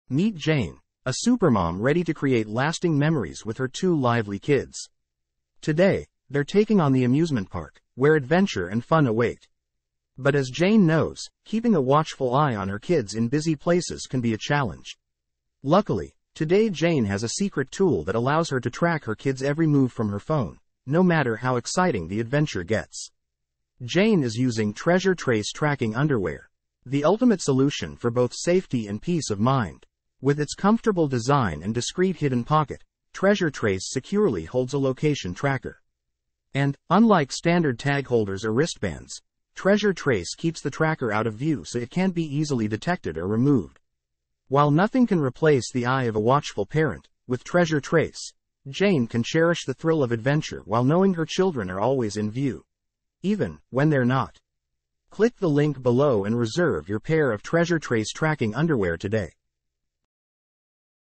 Introducing Treasure Trace Underwear
TreasureTrace Underwear are designed to comfortably hold a mini tracking device with a hidden, tamper-resistant pocket sewn into the inner waistband. The perfect, modern solution for keeping track of your child.